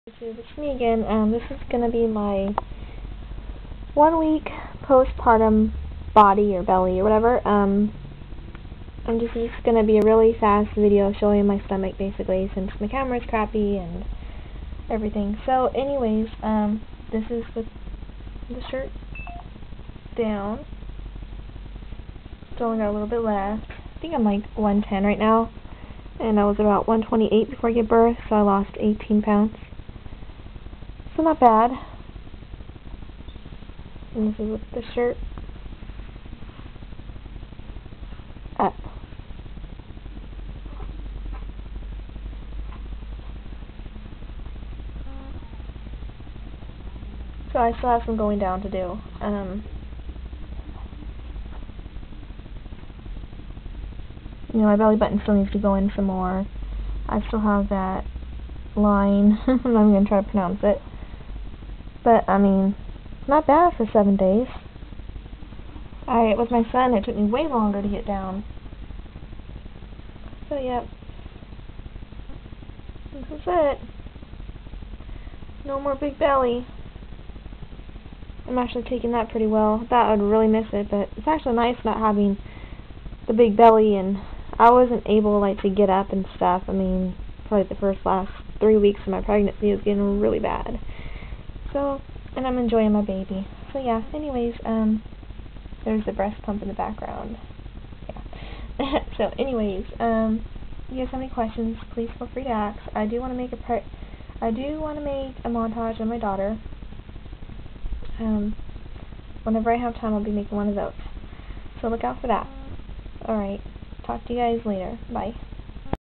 1 week after birth body
After 37 weeks of pregnancy & gaining 28lbs i am now 110 a week after birth i couldnt be any happier with how my body looks i am slowly getting back to my pre preggo body only 12lbs until i am back to my pre preggo size = )